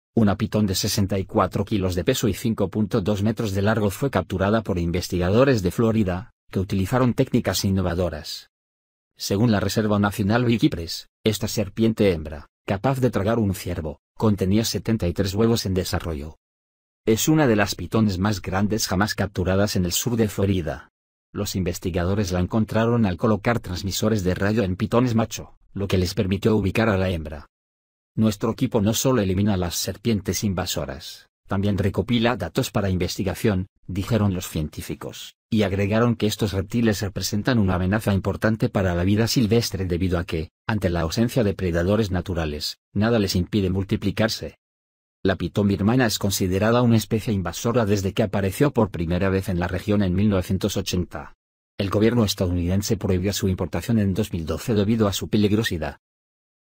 Capturan pitón de más de 5 metros de largo
Una pitón de 64 kilos de peso y 5.2 metros de largo fue capturada por investigadores de Florida, que utilizaron técnicas innovadoras.

Según la reserva nacional Big Cypress, esta serpiente hembra, capaz de tragar un ciervo, contenía 73 huevos en desarrollo.
Es una de las pitones más grandes jamás capturadas en el sur de Florida. Los investigadores la encontraron al colocar transmisores de radio en pitones macho, lo que les permitió ubicar a la hembra.

Nuestro equipo no solo elimina a las serpientes invasoras, también recopila datos para investigación", dijeron los científicos, y agregaron que estos reptiles representan "una amenaza importante para la vida silvestre" debido a que, ante la ausencia de predadores naturales, nada les impide multiplicarse.

La pitón birmana es considerada una especie invasora desde que apareció por primera vez en la región en 1980. El gobierno estadounidense prohibió su importación en 2012 debido a su peligrosidad.